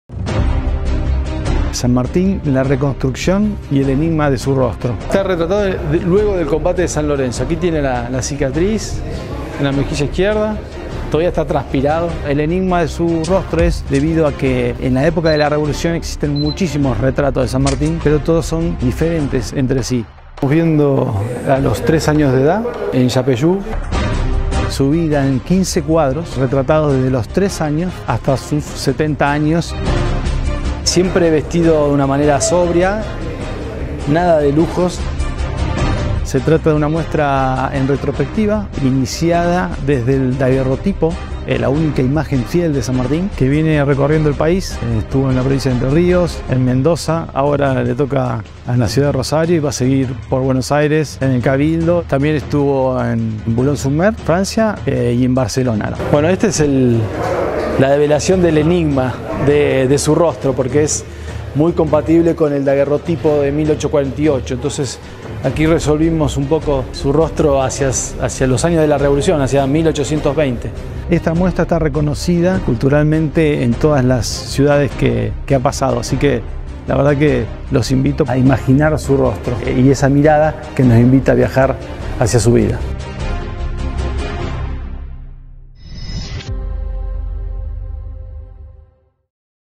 MUESTRA “SAN MARTÍN: RECONSTRUCCIÓN Y ENIGMA DE SU ROSTRO”
El autor es el artista gráfico Ramiro Ghigliazza. Se encuentra disponible para ser visitada hasta el 11 de noviembre en el Concejo Municipal.
  Se reconstruye la imagen del prócer José de San Martín en realidad digital desde los 3 a los 70 años, pasando por su vida en Yapeyú, hasta su muerte en Boulogne Sur Mer, Francia, con un exhaustivo trabajo de investigación basado en los testimonios de sus contemporáneos y en la única fotografía existente del general tomada en el año 1848 en París, cuando él mismo tenía 70 años de edad.